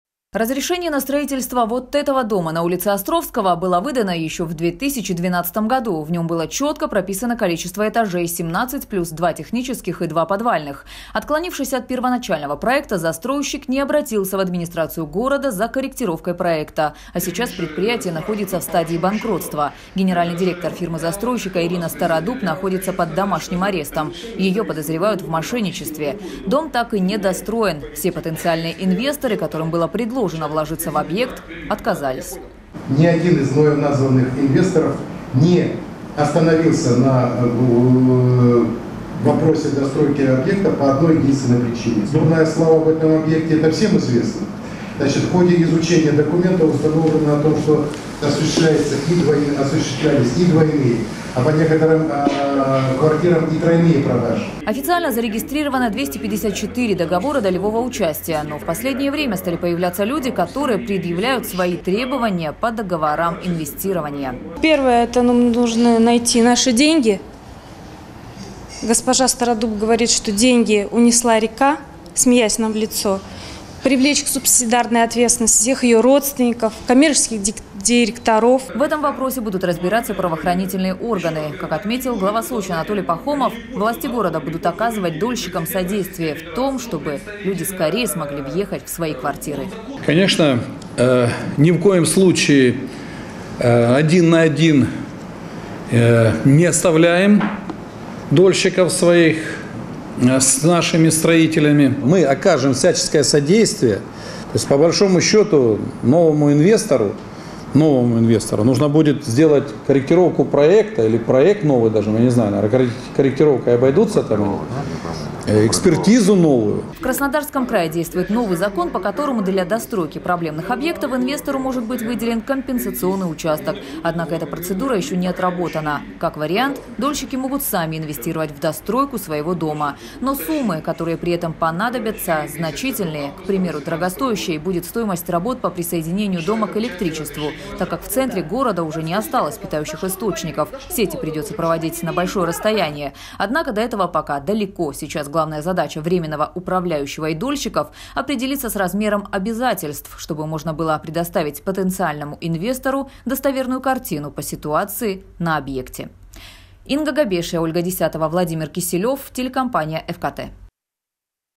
Для проблемного долгостроя в самом центре Сочи ищут инвестора. Новости Эфкате
Глава Сочи встретился с дольщиками ЖК «Абсолют». Ситуация с этим объектом непростая. Недострой находится в самом центре города и уже больше шести лет дольщики не могут получить ни квартир, ни назад своих денег за квадратные метры.

Разрешение на строительство дома на ул. Островского было выдано еще в 2012 году. В нём было чётко прописано количество этажей - 17 плюс два технических и два подвальных. Отклонившись от первоначального проекта, застройщик не обратился в администрацию города за корректировкой проекта. Сейчас предприятие находится в стадии банкротства. Генеральный директор фирмы-застройщика Ирина Стародуб находится под домашним арестом. Её подозревают в мошенничестве. Дом так и не достроен. Все потенциальные инвесторы, которым было предложено вложиться в объект, отказывались.